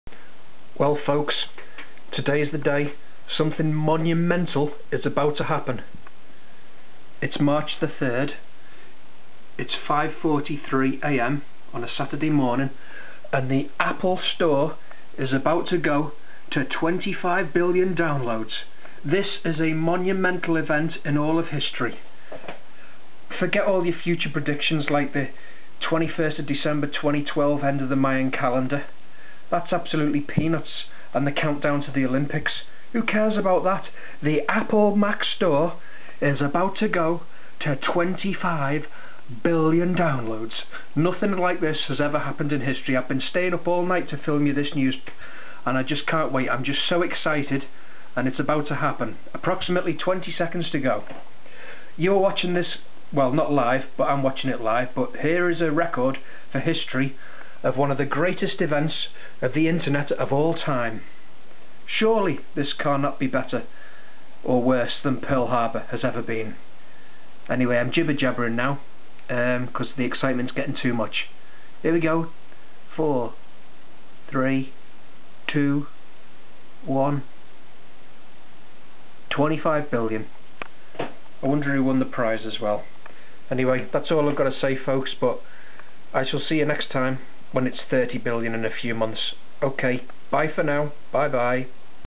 Apple App Store Hits 25 Billion Downloads
Don't worry folks, if you missed it I stayed up all night and recorded it as mark of respect to all you tech geeks out there.